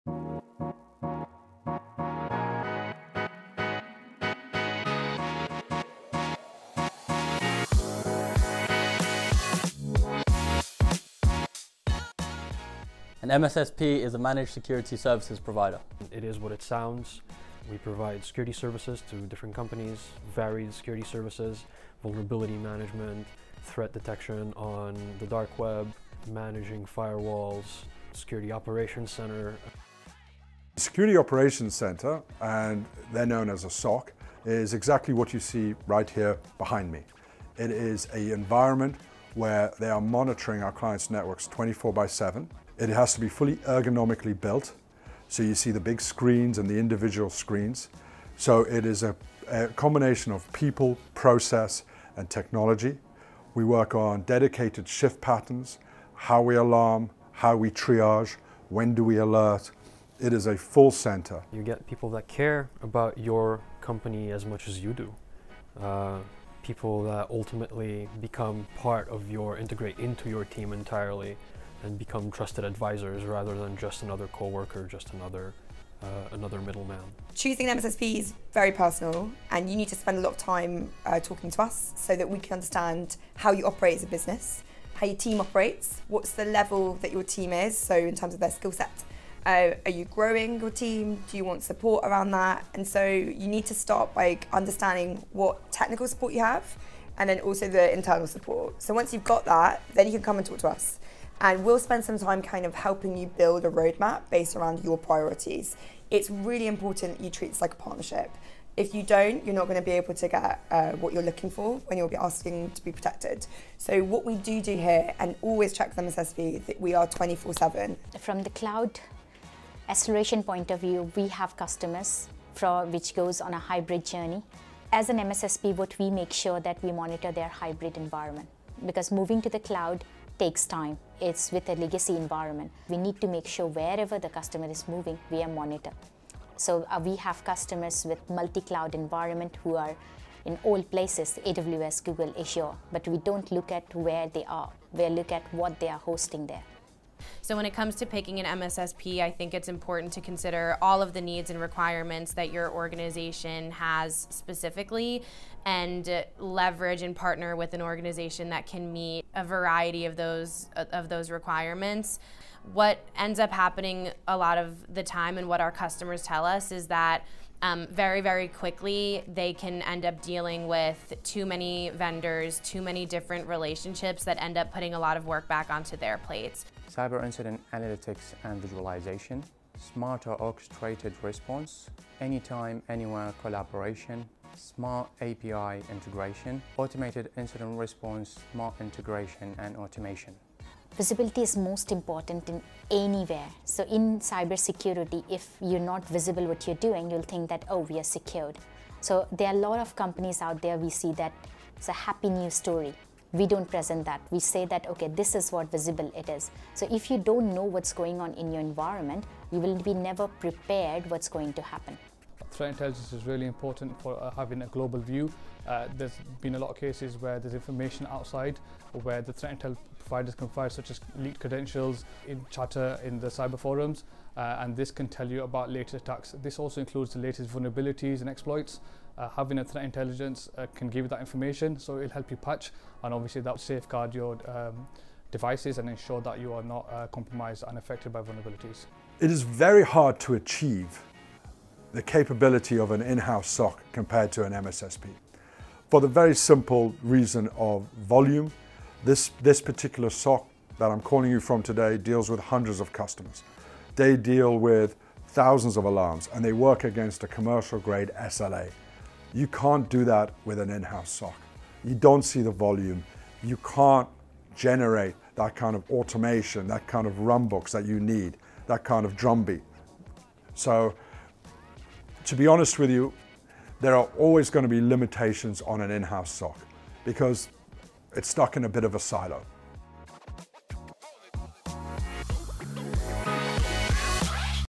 Cyber Security Insights - What is an MSSP?
What is an MSSP (Managed Security Service Provider)?

Watch this video to understand how a Managed Security Service Provider (MSSP) functions. How to choose an MSSP. What is important to you when selecting your provider. What goes into creating a Security Operations Center (SOC), and how a security team works together to detect and respond to vulnerabilities impacting your business.

SecurityHQ is a Global MSSP that monitors networks 24/7, to ensure complete visibility and protection against your cyber threats.

Speakers:
SecurityHQ's CEO, Feras Tappuni
Nazam Khan,  Senior Cyber Security Manager
Ali Al-Rubaye, Senior Cyber Security Manager
Claudia Banks, Head of Sales EMEA & APAC
Samantha Hunt, Regional Sales Lead
Renz Crammond, Cyber Security Manager
Priyanka Agarwal, Head CSM & Data Analytics
Tom Tzivan, Cyber Security Manager

For more information about SecurityHQ or Managed SOC Services please

00:00 What is an MSSP?
00:31 What is a Security Operation Center (SOC)?
01:02 What are the benefits of using an MSSP?
01:17 How to choose an MSSP?
02:27 Tips for choosing an MSSP
04:00 In-house SOC vs outsourcing SOC